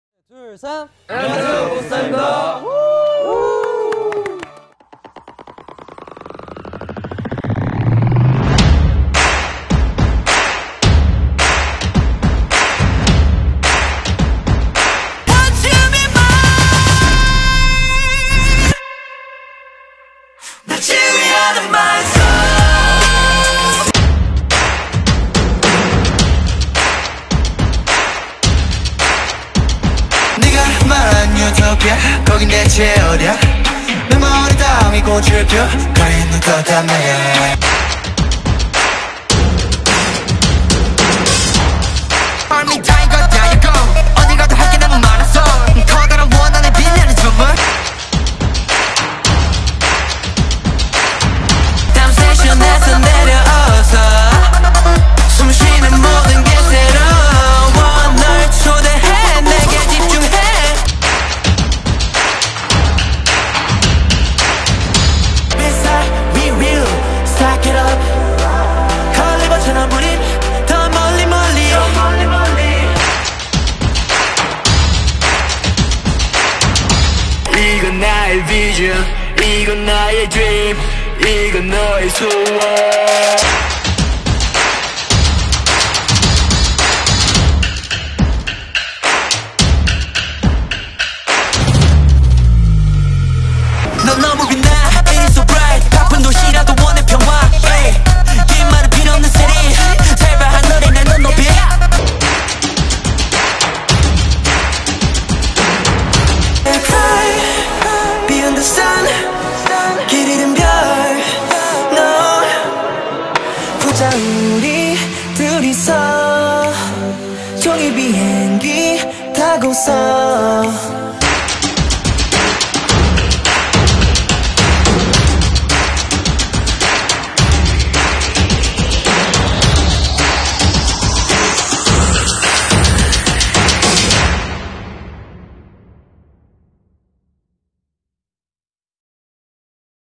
Introducing GHOST9 | Member Names
Are you trying to learn the members of Ghost9? We hope this video helps! Let us know if you find this fun, fast-paced introduction useful and if we should make more like it for other groups!